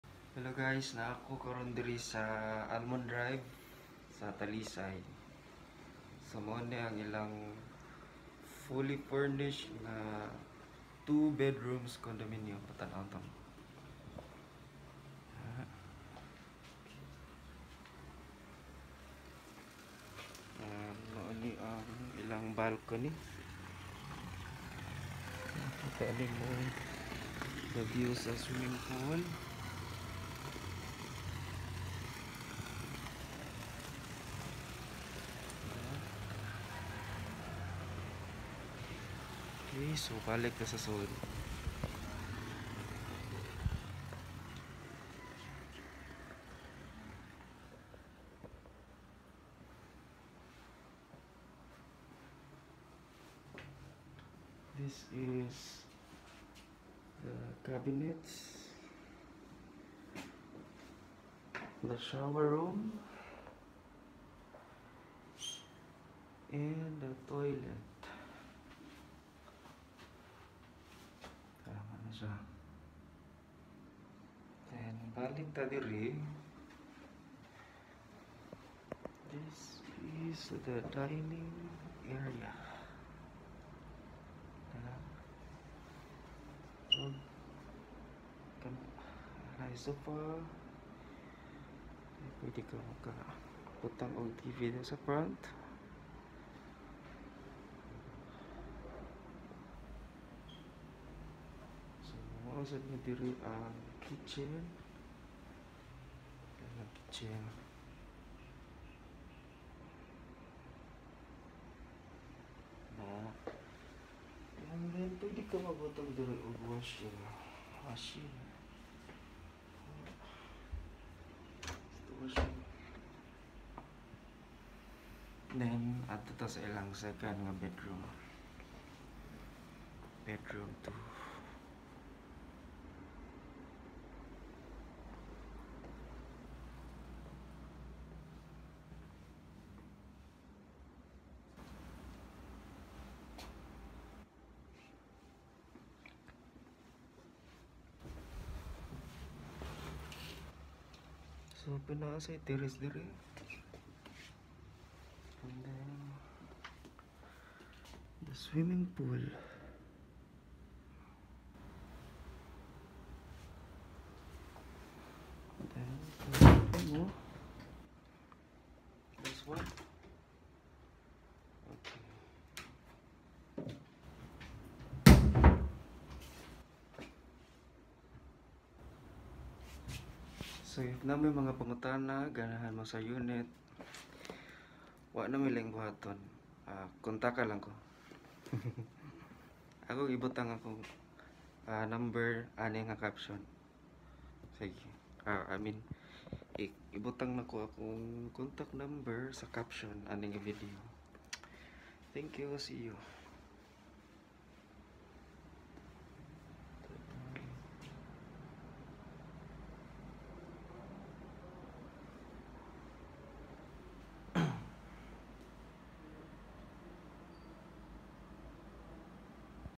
Almond Drive Walk Up Condominium In SRP
Walk up condominium nga naa ra sa SRP ug accesible sa Sm seaside, el curso ug uban pa…

‼️‼️‼️Avail na‼️‼️‼️

Walay Hassle sa requiremnts…2 valid IDs lang payts na. Matik dayon ka maka reserv sa unit…

Available Units:
💎Studio Type
💎1 BEDROOM
💎2 bedrooms

*FREE PARKING PA JUD… asa paka …

Kato diay cgeg ask nako sa chat… dia na akoa najud gi adto ang site ug nindot kaayo ang location…. RESERVE NA !